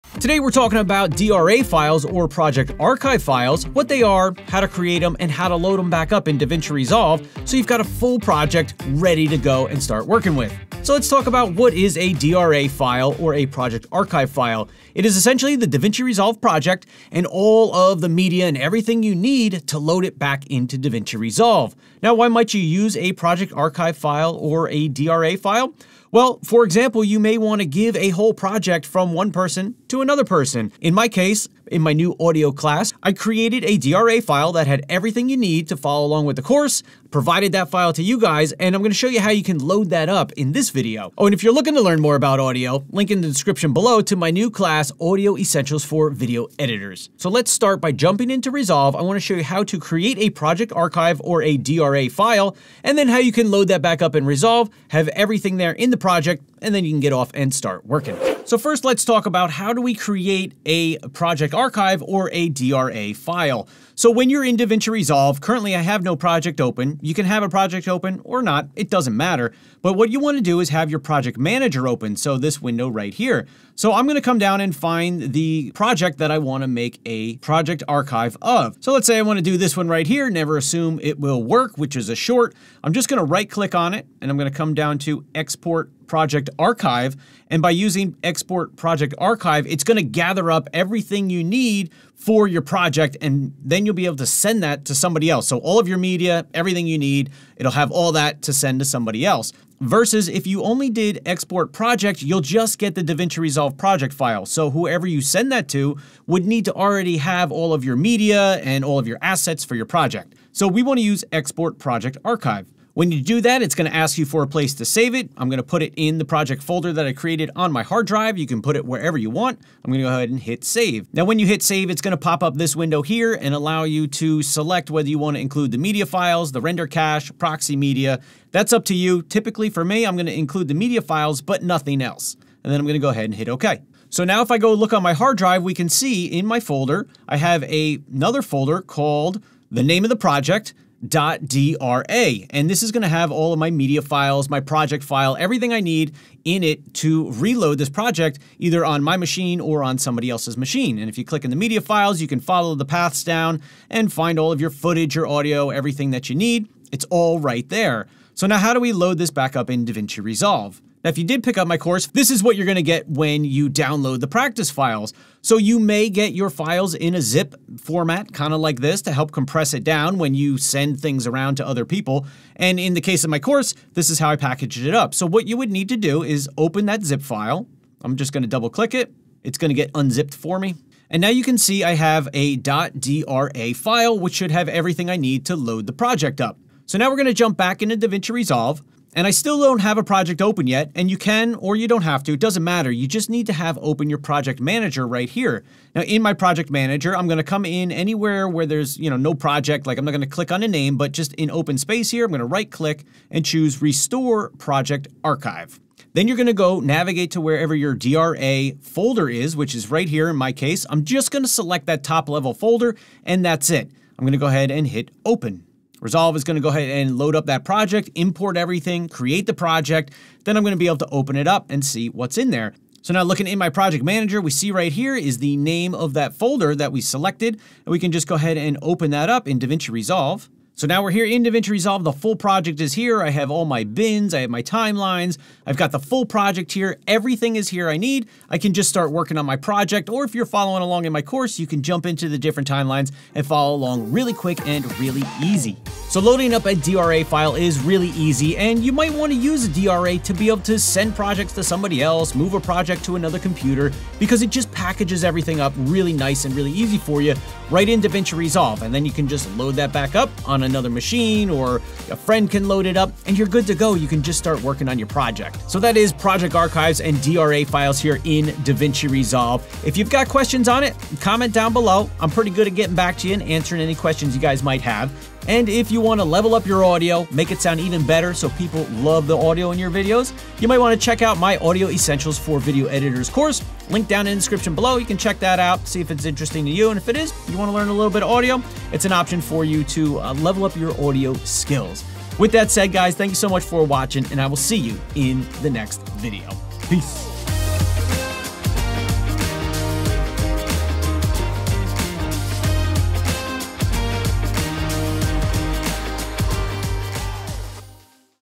CREATE & LOAD .dra Files (Project Archive) in DaVinci Resolve 18 and Why
Have you ever seen a .dra file for DaVinci Resolve 18 but you don't know what to do with it?  I'll tell you WHY they are awesome, HOW to Create a .dra and HOW to Load a .dra file.

🔥 My Audio Course! 🔥

0:00 What is a .dra File or Project Archive
0:21 Why us .dra?
0:58 How to Create Project Archive (.dra)
1:39 Export Project Archive vs Export Project
2:00 .dra Project Archive Options
2:38 Load .dra in DaVinci Resolve
4:05 Final Thoughts